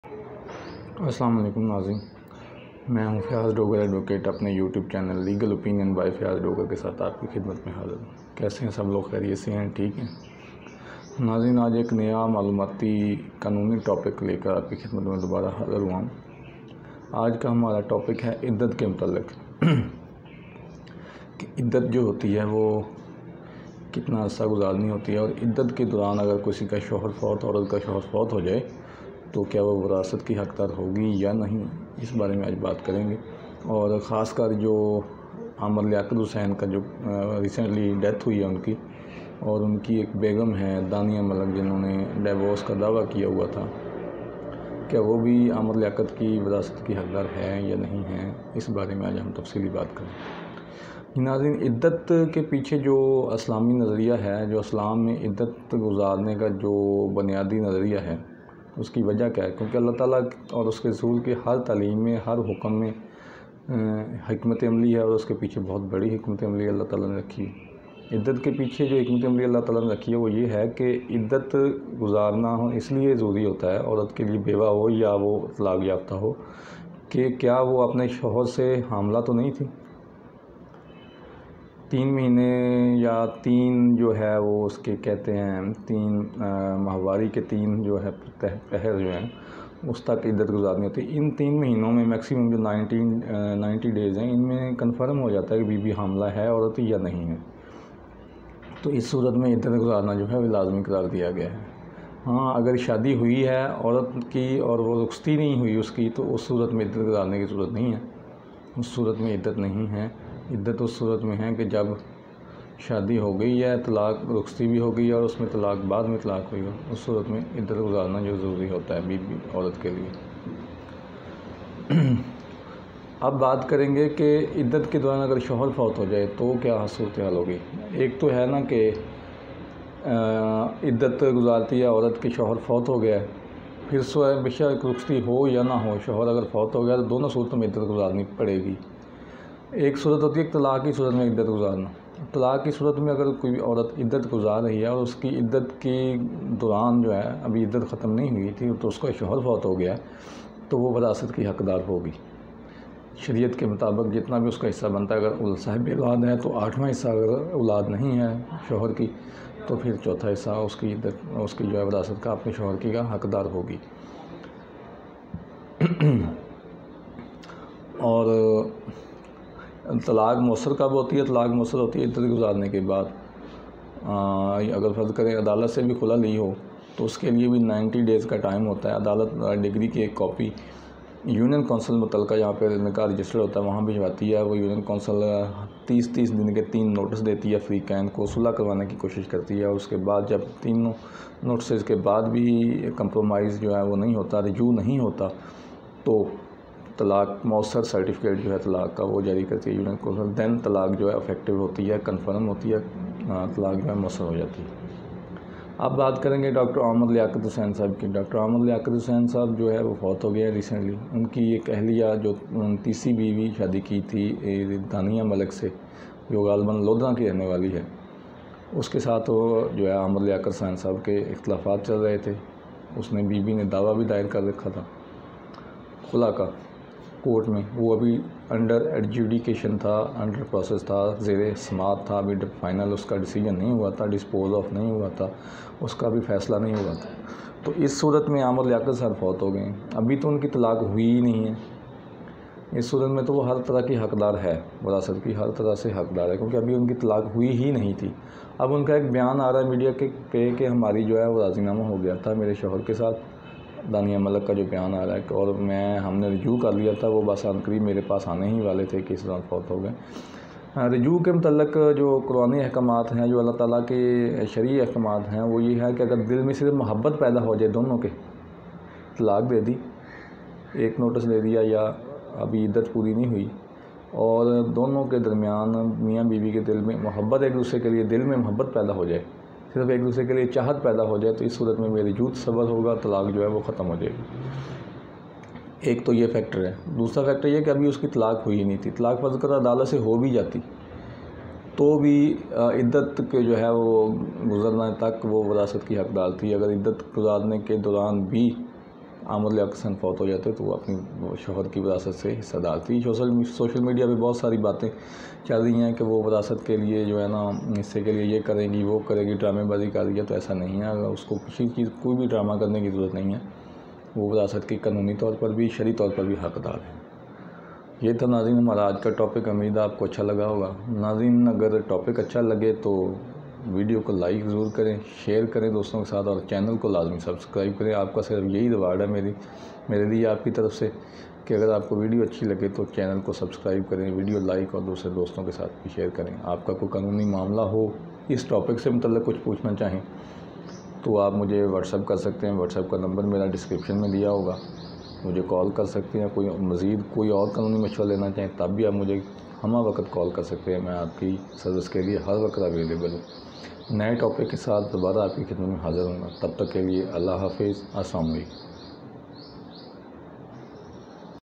Iddat how many days have to observe a divorced lady in Islam. A lecture by Advocate Fiaz dogar
Iddat how many days a divorced lady have to observe
Kia Dania Malik Apne husband Amir Liaqat Hussain  ki warasat ki haqdar ha
A lecture by Advocate Fiaz dogar
office at Civil Court Haroonabad
District Courts Faisalabad